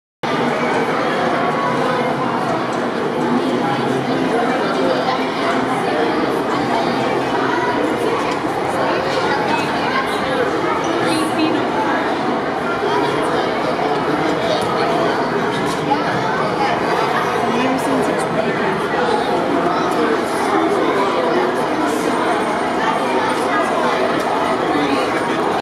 17 04 Shedd Aquarium 1002
Raising Amazon exhibit at the Shedd Aquarium in Chicago.
Polka-dot catfish